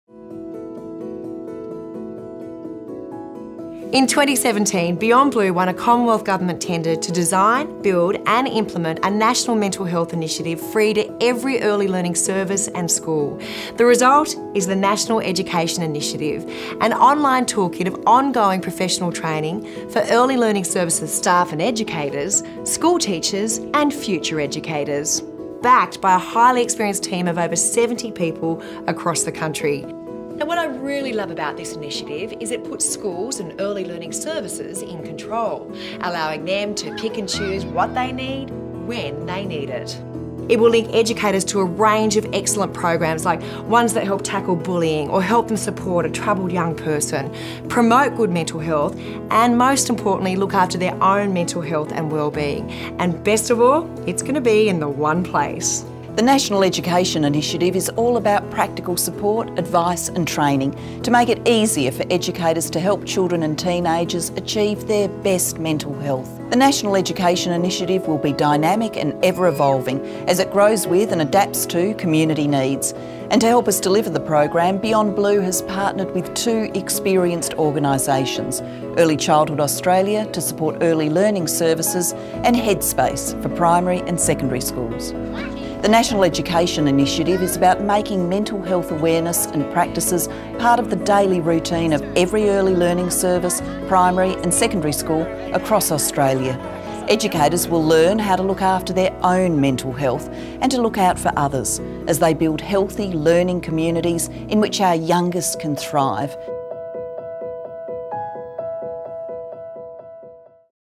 National Education Initiative
Learn more about the National Education Initiative from The Hon Julia Gillard AC, Beyond Blue Chair and Johanna Griggs, Beyond Blue Director and Chair of the National Education Initiative National Advisory Council.